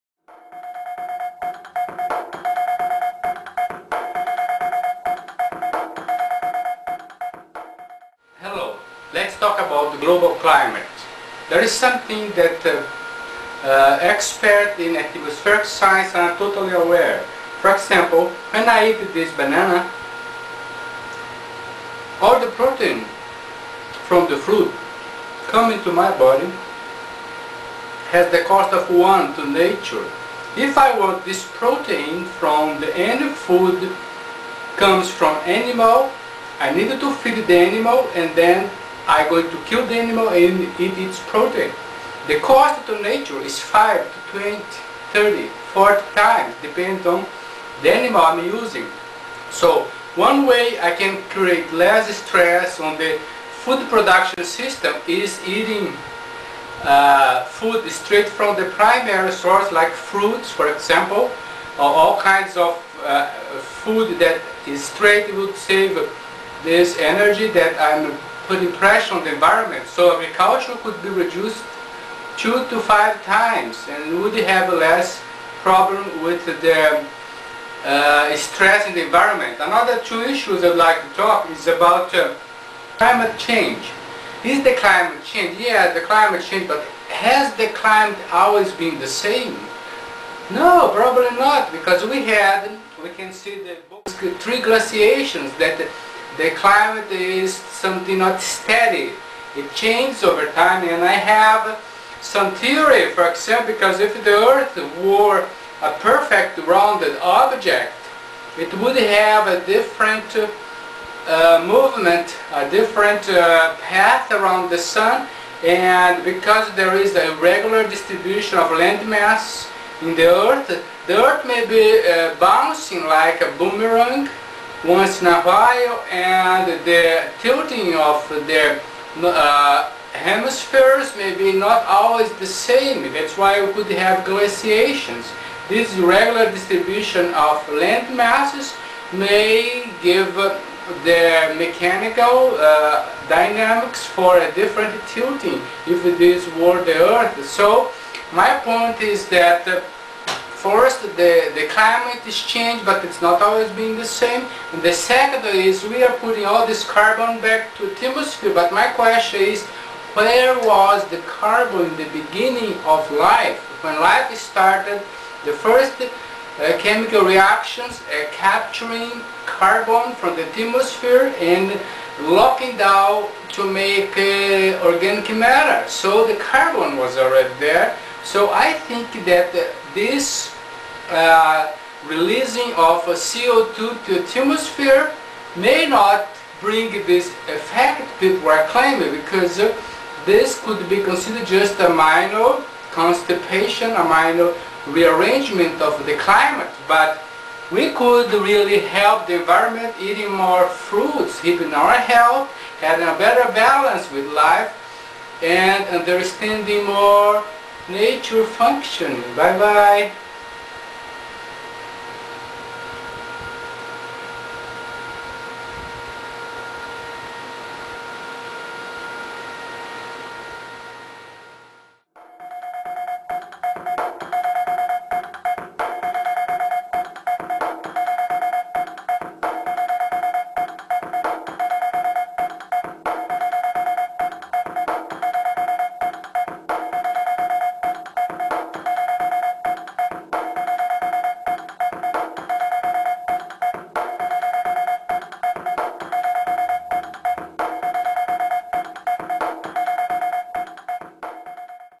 101 Tubarc - Climate Change and Carbon Cycle
The climate has always been changing and when life started no carbon was locked up in the organic matter.